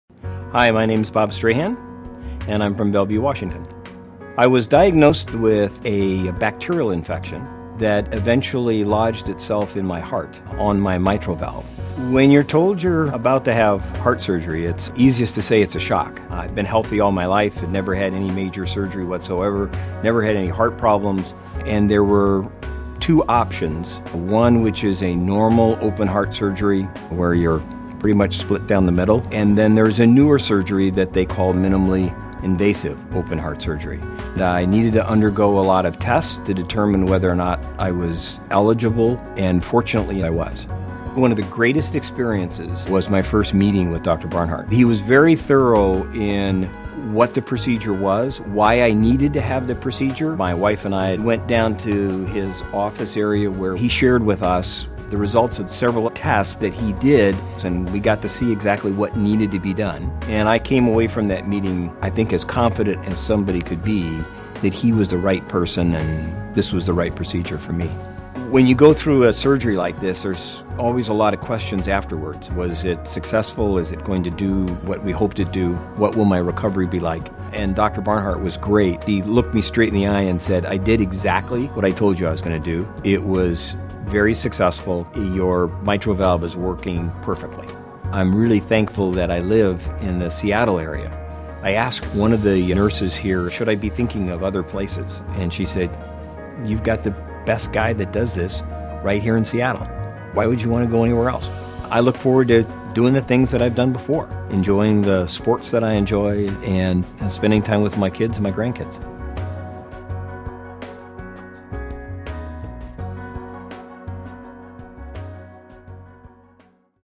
A patient's experience with heart surgery
Bob talks about his experience with minimally invasive heart surgery after he was given the options of traditional heart surgery or minimally invasive surgery.